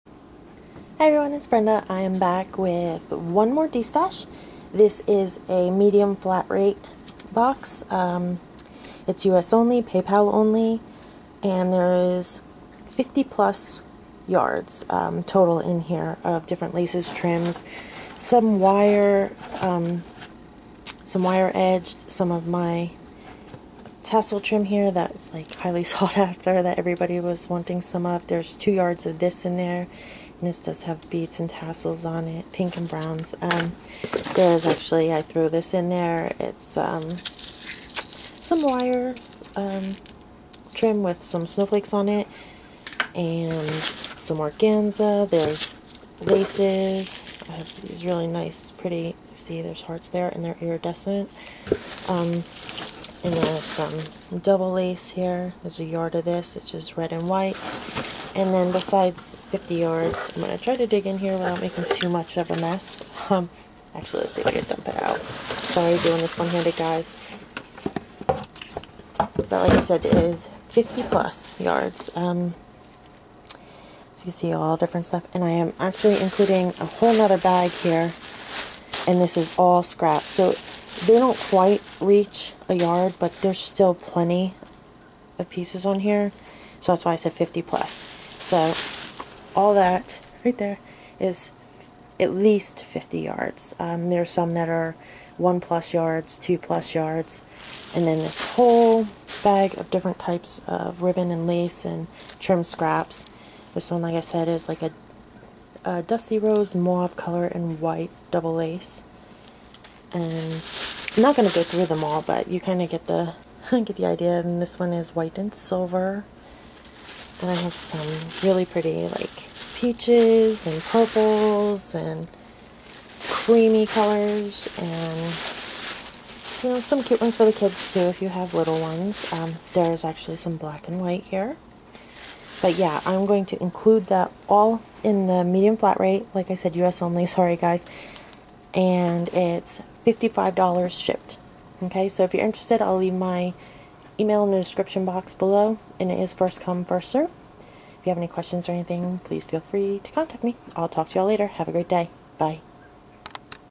Destash grab box of lace trim and ribbons SOLD!
correction large flat rate box of mixed ribbons laces and trims

50 plus yards and a bag of scraps too

$55.00 shipped

TFW!

Here are a few more places you can join me on the web!

LittleRaven1

Ravelry-

LittleRaven1